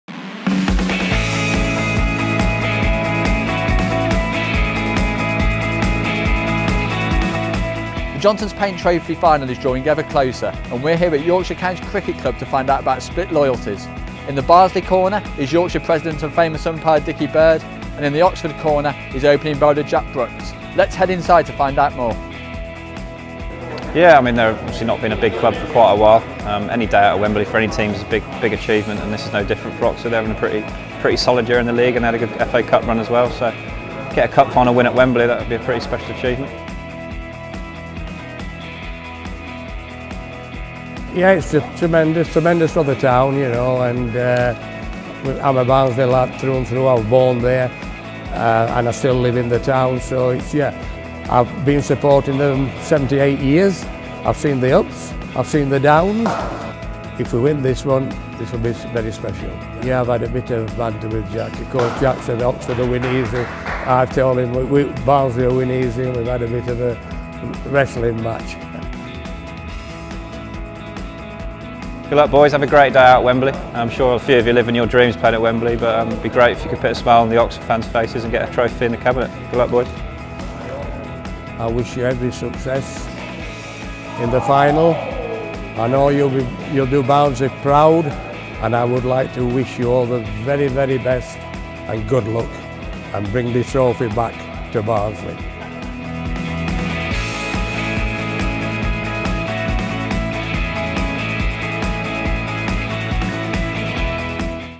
Split loyalties at Yorkshire County Cricket Club
It's a case of split loyalties at Yorkshire County Cricket Club, where Barnsley fan Dickie Bird and Oxford United fan Jack Brooks face off ahead of the Johnstone's Paint Trophy Final on April 3.